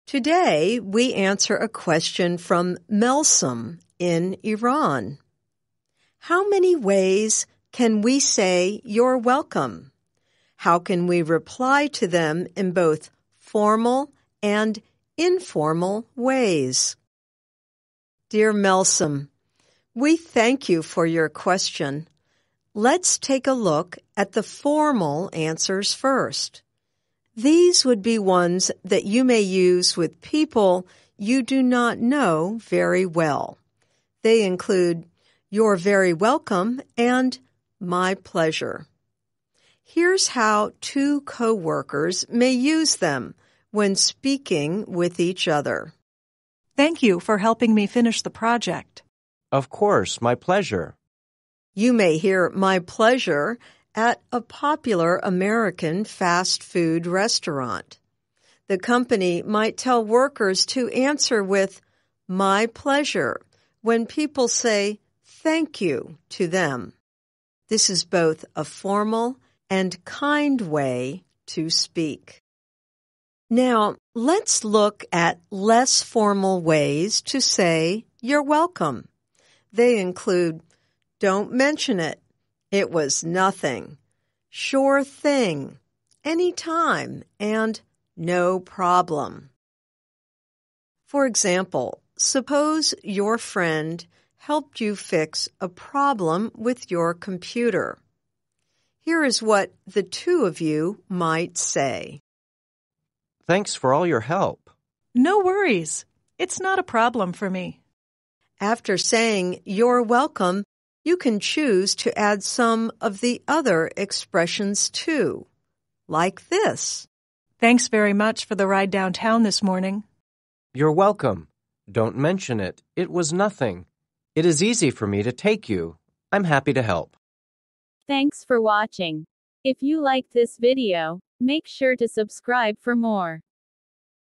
Many Ways to Say "You’re Welcome" | English Phrase
In this video lesson, you will learn many ways to say you're welcome. When you learn to speak a new language whether English or a different language, you should pay attention to the most common phrases that are used in that language. Besides common vocabulary, common phrases are one of the most important factors that decide whether or not you can speak a new language well in a short period of time. @learnenglishwithnanncy